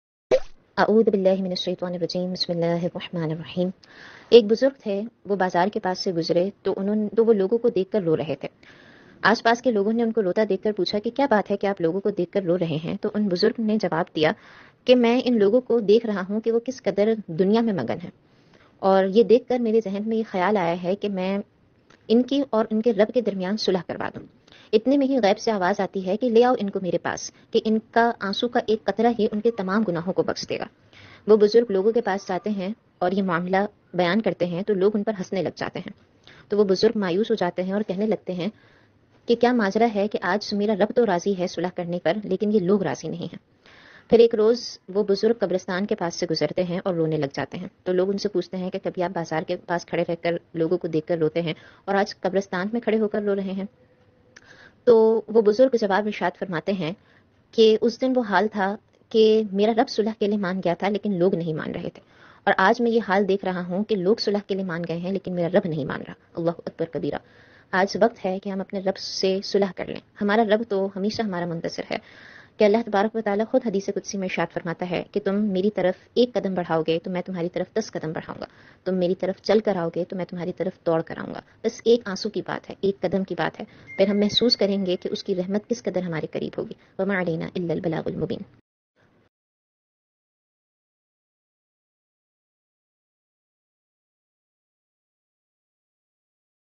rab se sulah |turn to Allah|Allah ko razi karna| Raah e islam
Turn to Allah before you return to Allah!!
Rab se sulah karlo phir kisi ki narazgi ka asar nahi hoga .. woh razi tou sab razi,woh naraz tou sab bekar!!